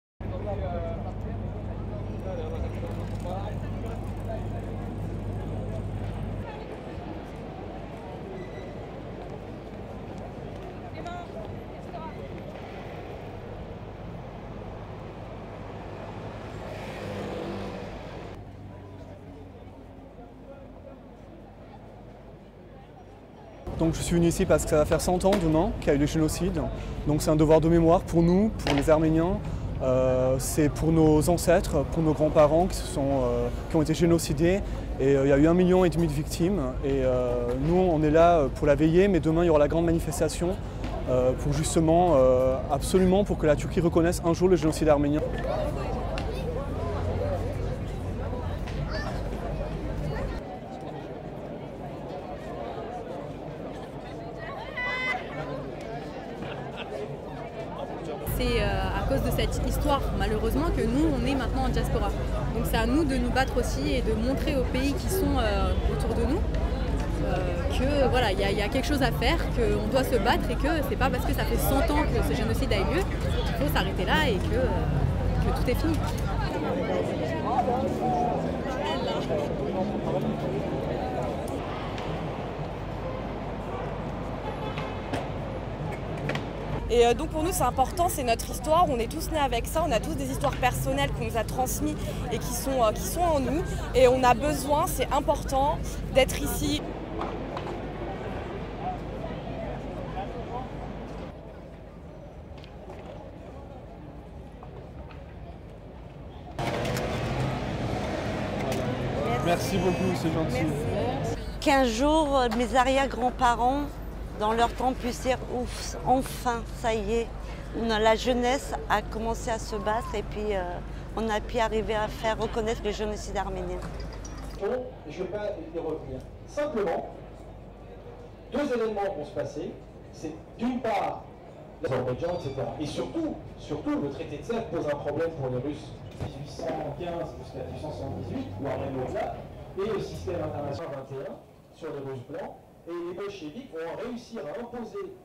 French Armenians hold vigil to remember victims
SHOTLIST
AP TELEVISION - AP CLIENTS ONLY
Paris - 23 April 2015
1. Statue in Place de la Republique
2. Tent put in place for the event with words reading (French) "Armenian genocide of 1915" written on it
3. Side of tent with the slogan reading (French) "No to Holocaust denial" written on it
4. People looking at map of Armenia
5. SOUNDBITE (French) Arthur Arakeloff, Vigil participant:
"I came here because tomorrow will be 100 years since the genocide happened. It's a duty for us, for Armenians. It is for our ancestors, for our grandparents who were killed, there were 1,5 million victims, we are here for the event, but tomorrow a big protest will take place to make sure that Turkey will recognise the Armenian genocide."
6. Arthur wrapped in Armenian flag
7. Woman holding Armenian flags
8. SOUNDBITE (French) Gayane Minassian, Vigil participant:
"It is because of this history unfortunately that we are a diaspora. That's why we have to fight and to show to the countries around us that something can be done. And just because it happened a hundred years ago doesn't mean that we should stop fighting."
9. Woman's face
10. Young people
11. SOUNDBITE (French) Marie Kalaghabian, Vigil participant:
"To us it is important, it is our history, we were all born with it, we all have personal histories that have been transmitted, and that are still in us, and we need it - it is important to be here."
12. Woman in front of a board with the history of the killings written on it
13. Corner of banner reading (French) "Genocide"
14. People taking badges to put on their jackets
15. SOUNDBITE (French) Maggie Canli, Vigil participant:
"(I wish) that one day, my great grandparents in their tomb can say - finally, it's done - the youth have begun to fight and we managed to make the Armenian genocide recognised."
16. People inside tent listening
17. People standing and listening
18. Woman sitting down and listening
19. Logos of the event
STORYLINE
French ethnic Armenians held a vigil in Place de la Republique, central Paris, on Thursday on the eve of the international commemoration of the centenary of the killing of 1.5 million Armenians by Ottoman Turks around the time of WW1.
100 years later they want the killings recognised as genocide.
Many scholars have called the Armenian deaths the first genocide of the 20th century.
However Turkey denies that the deaths amounted to genocide, saying the number has been inflated and that those killed were victims of civil war and unrest.